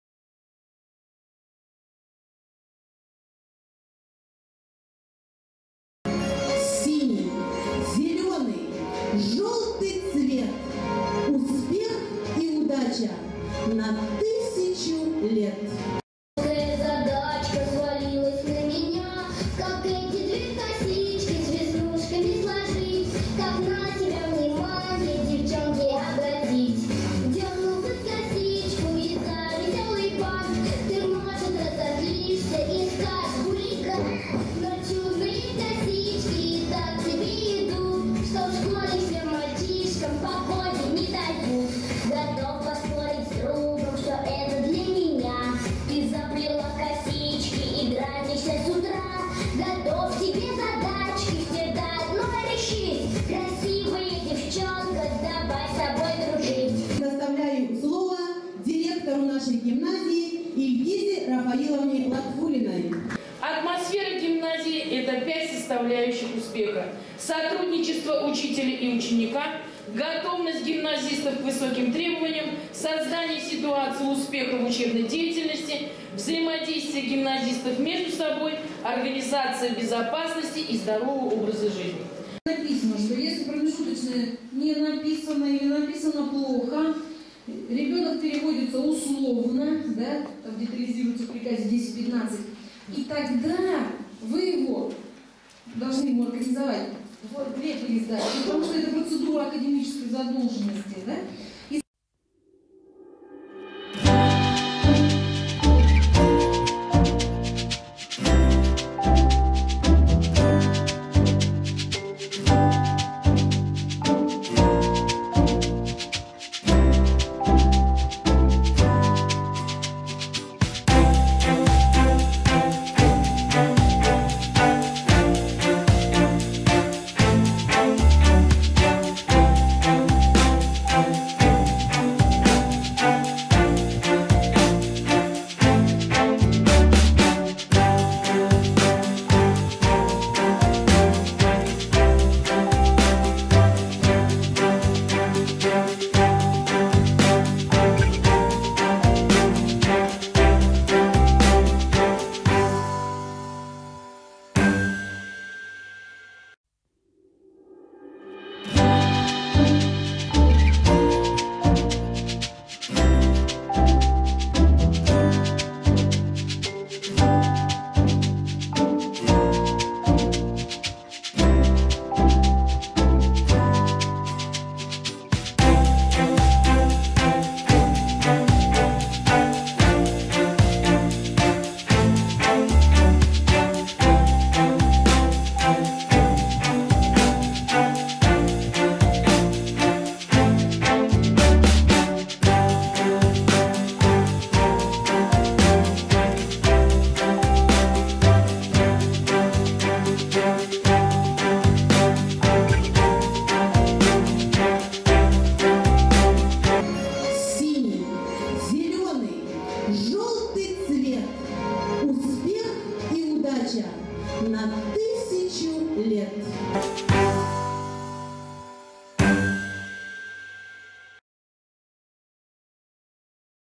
Гимназия учит учителей
Областной методический семинар учителей в гимназии  Линево